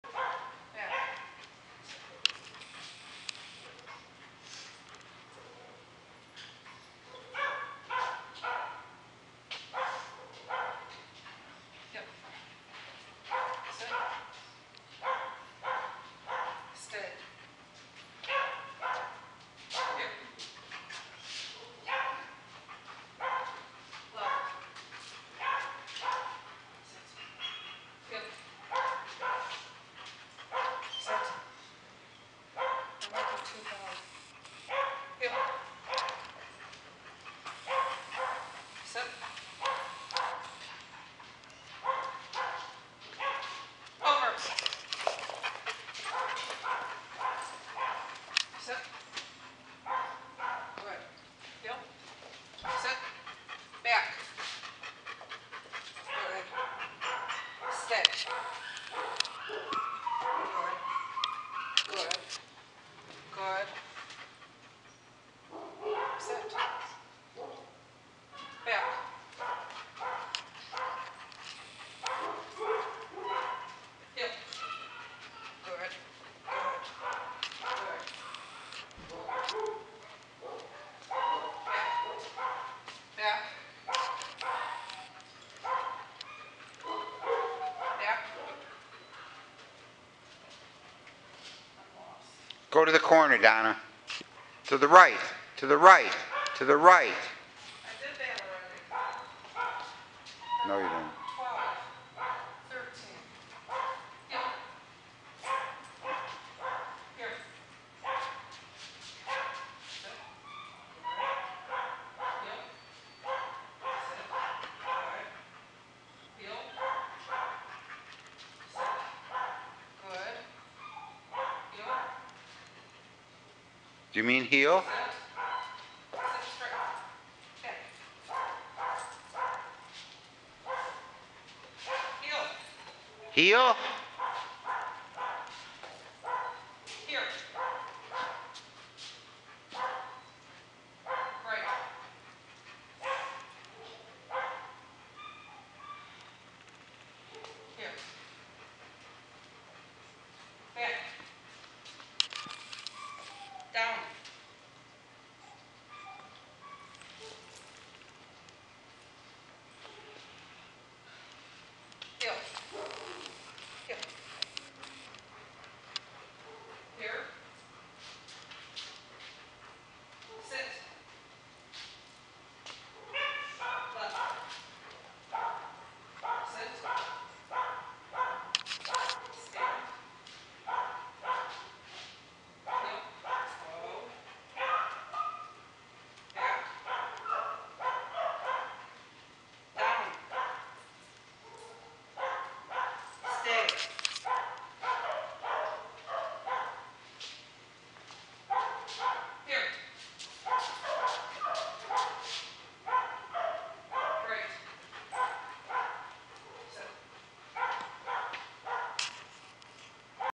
Allan Ross Dog Training
Donna and Ely  practice a Cynosport Level 2 Rally Obedience course in the Canine College.  Concentration